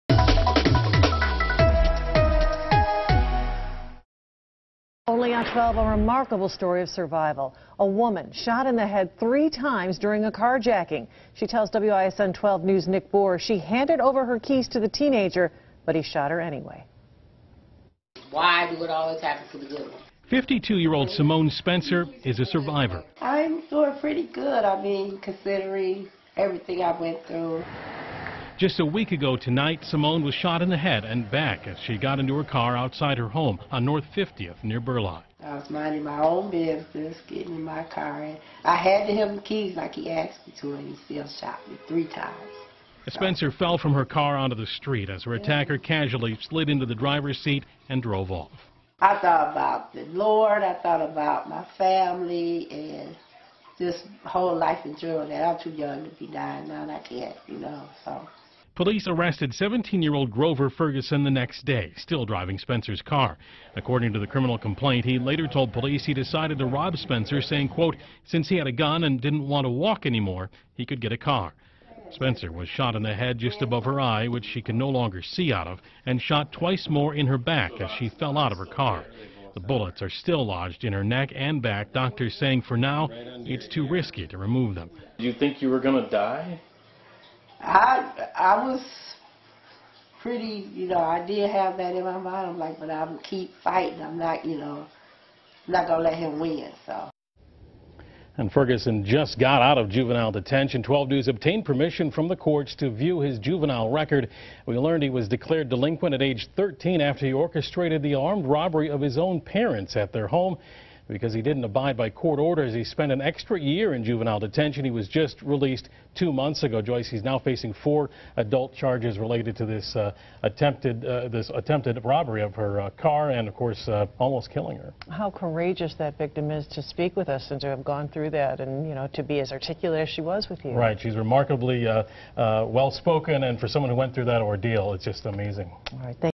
Woman survives being shot 3 times in carjacking
Samone Spencer, who was shot three times during a carjacking, is a survivor.